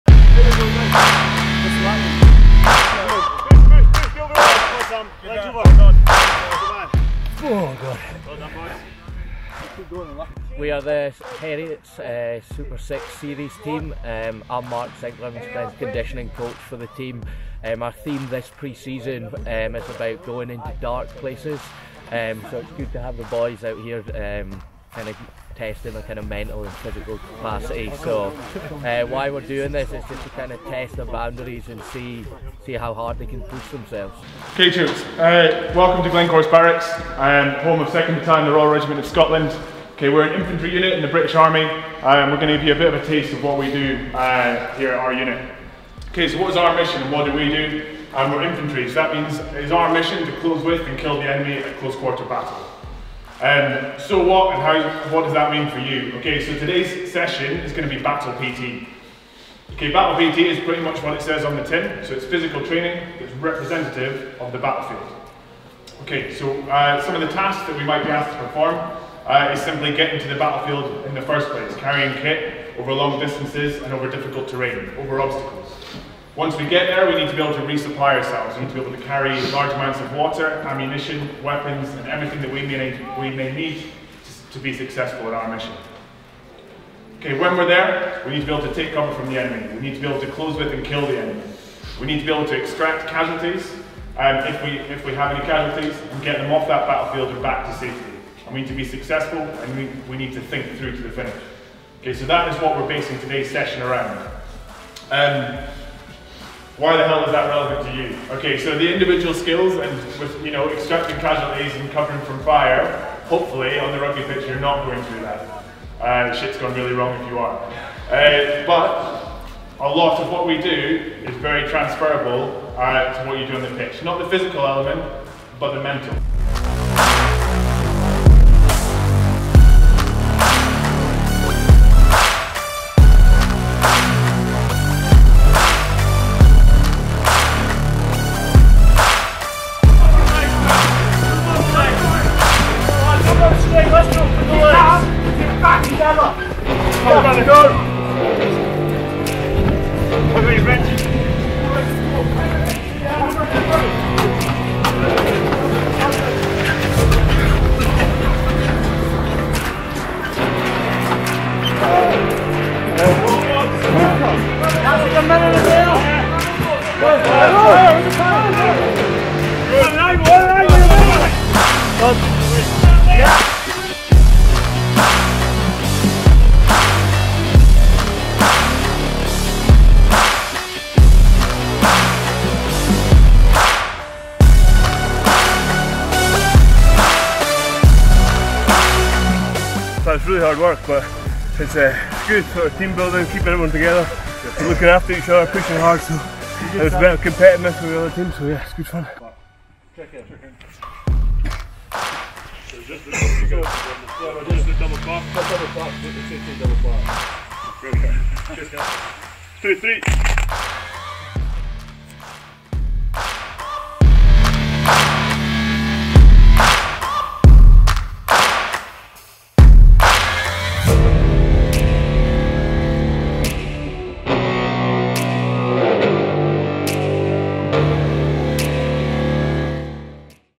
2 SCOTS Battle PT with Heriots Super Series Rugby Team
2nd Battalion The Royal Regiment of Scotland recently hosted the Heriot's Rugby Super Series team for an introduction to Battle PT.

Battle PT is designed to test a soldier’s body to the max. It is a way of recreating the physical demands of being in combat through tasks such as weighted runs and marches, casualty drags, and stretcher races.

Regular battle PT creates not only a high level of fitness but also mental robustness, as it is usually conducted outside on the training area regardless of the weather, instead of indoors in a warm gym.

Battle PT is always conducted as a unit, not as an individual. It is down to your fellow soldiers to make sure everyone succeeds and makes it to the finish line, even if some must push themselves a little harder than others.

At the end of the day, it’s that well known saying; ‘Train hard, fight easy’.